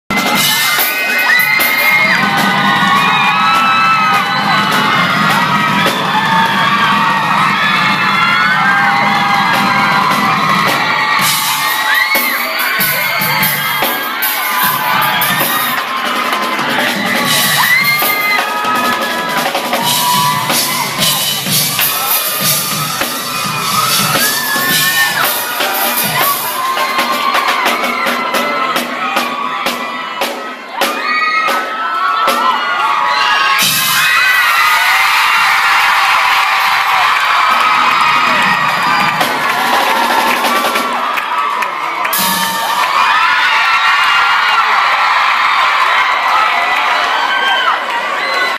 Justin Bieber Drum solo Live @ The Apollo Theater 18.06.12
Justin Bieber Drum solo Live @ The Apollo Theater June 18th 2012 In Harlem, NYC